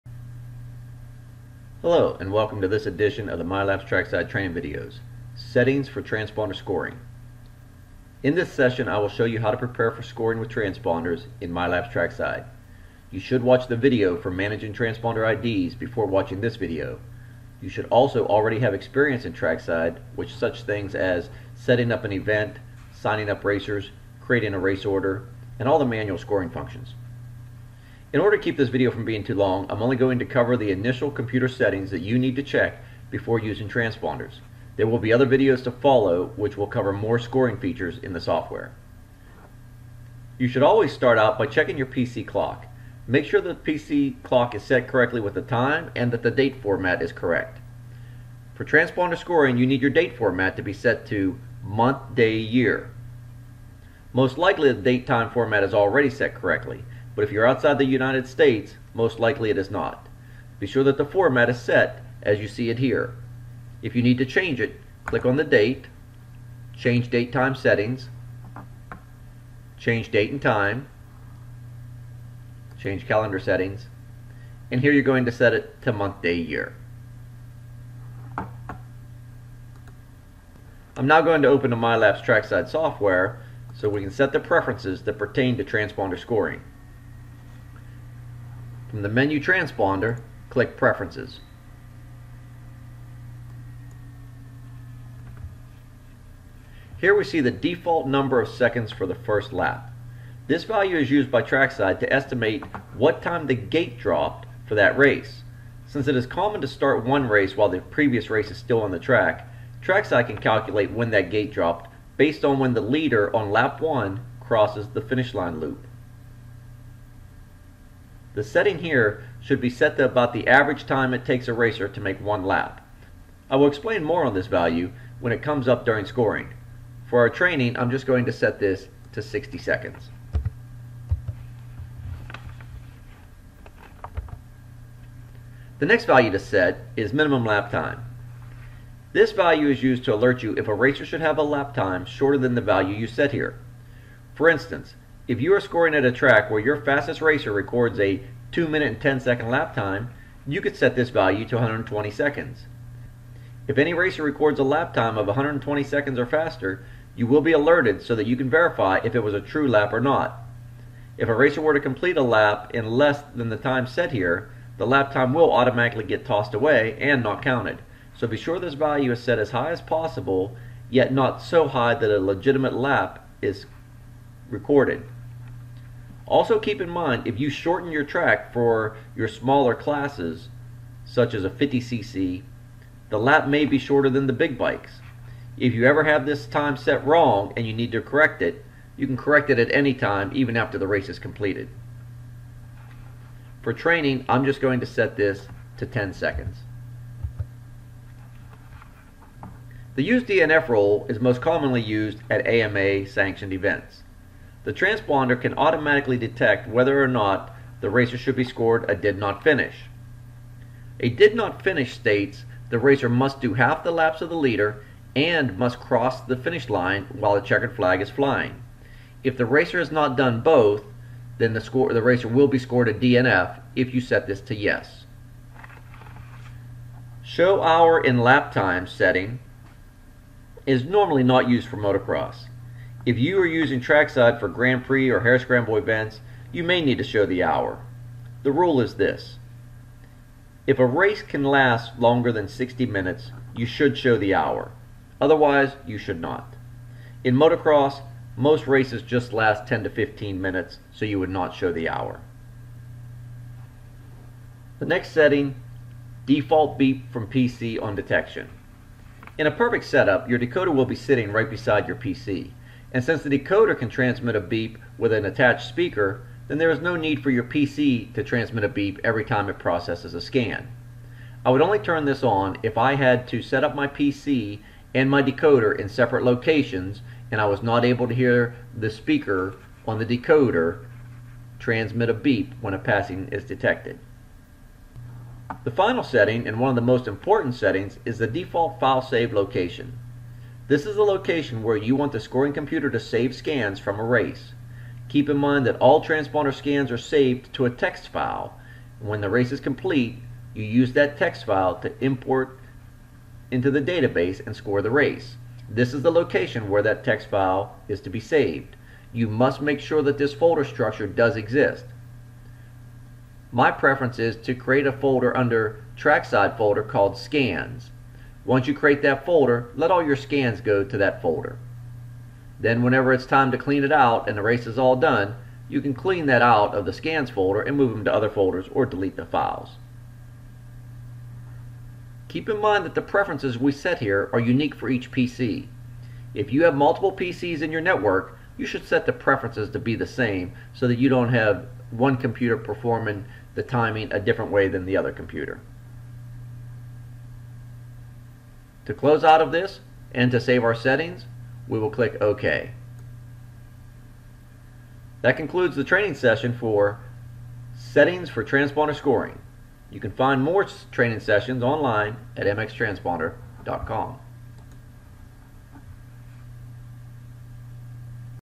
Intro to MylapsTrackside - Transponder Preferences - Video 4 of 5
Video created for Trackside users.  Trackside Software is designed specifically for motocross race organizers. The software will help you to manage the racers and their race results at your event by maintaining the data about the event and racers.  The software helps to manage the event by allowing you to define a race order and enter the results then creating reports to post.

This video is intended for Trackside users that are running Transponders for Scoring.  Shows and explains the Preference settings to use in Trackside.